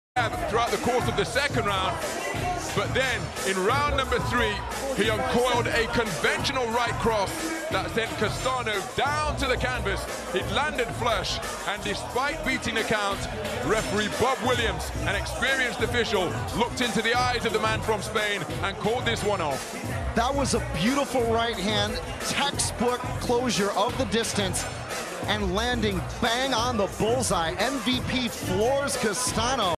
Michael Page debuts in boxing with knockout
The MMA star Michael Page def. by TKO Jonathan Castaño (3w-12l) in the first ever boxing match.

O lutador de MMA Michael Page estreou no boxe profissional com nocaute. Page, uma das estrelas do Bellator, nocauteou o espanhol Jonathan Castaño.

Vídeo by Dave.